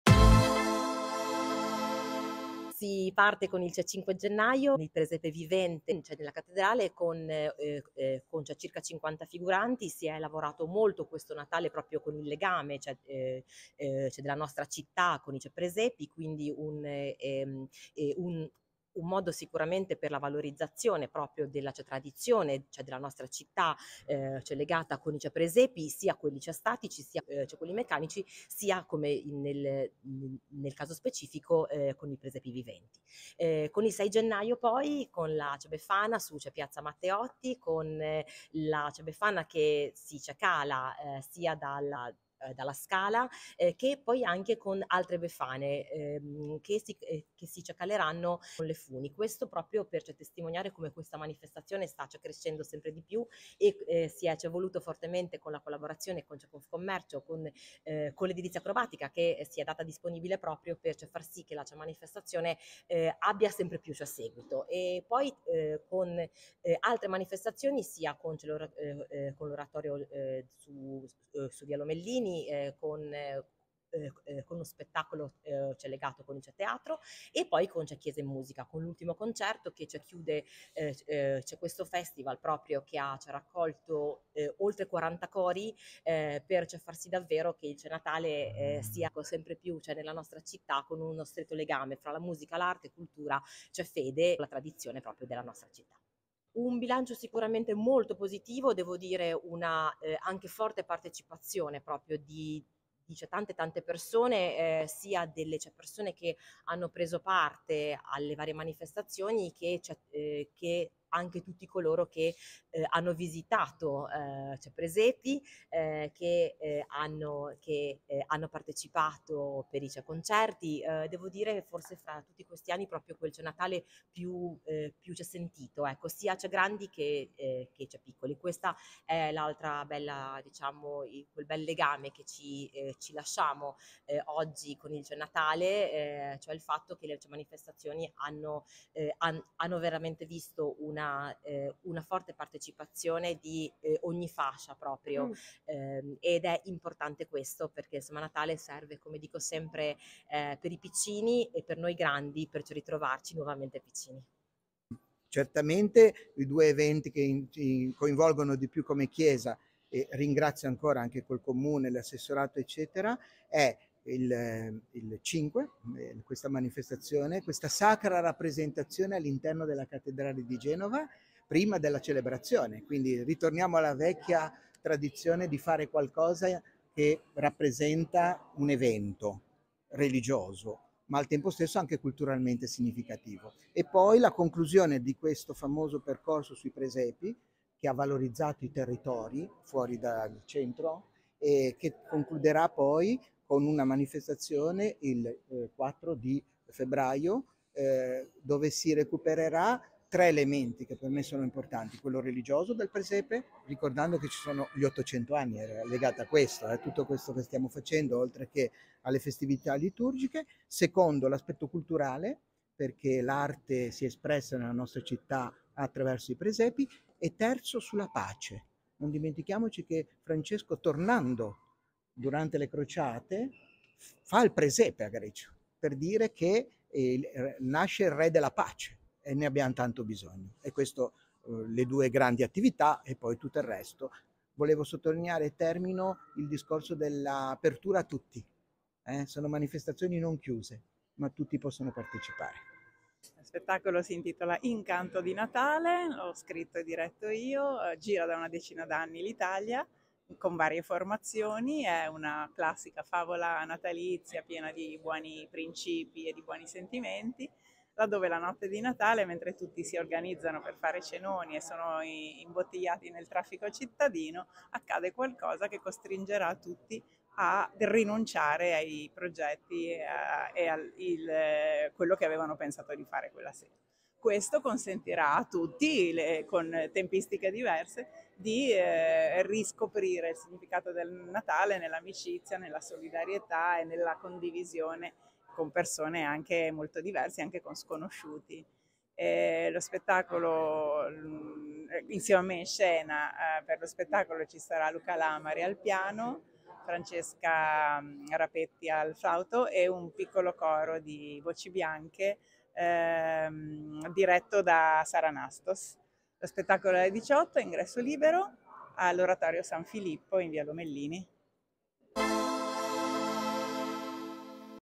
Presepe vivente, Befana acrobatica, musica e teatro: tutti gli appuntamenti dell’Epifania a Genova
Tanti eventi ‘tra sacro e profano’ per salutare le festività natalizie, tra cui il suggestivo presepe vivente nella Cattedrale previsto per venerdì 5 gennaio. Rimandato al 4 febbraio per maltempo il Corteo dei Magi